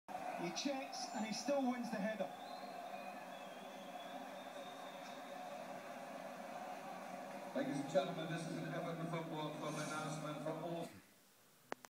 Celebrating Fellaini's goal
EFC v man united 20/8/12 was at the game spotted myself on the tele after!